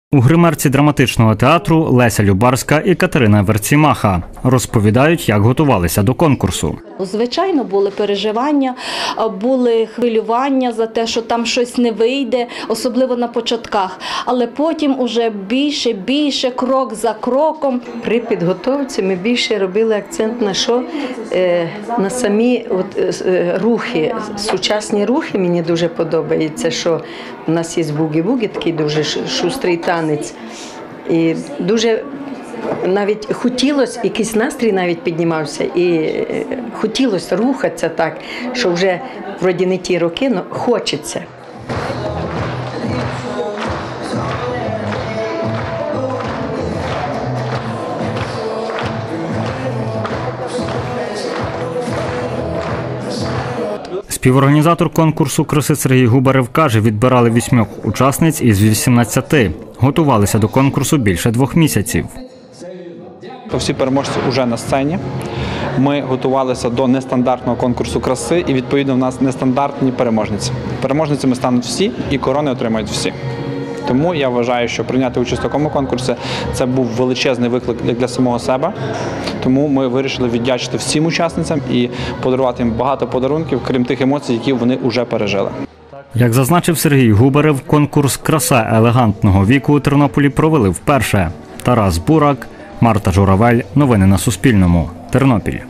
Жінки віком від 60 до 73-х років взяли участь у конкурсі «Краса елегантного віку»
8 учасниць віком від 60 до 73-х років взяли участь у конкурсі «Краса елегантного віку». Захід відбувся в Тернопільському обласному драматичному театрі. Розповів новинам співорганізатор Сергій Губарев. За його словами ініціювали захід, - Тернопільський міський територіальний центр та Молодіжна ініціатива «Файні внуки»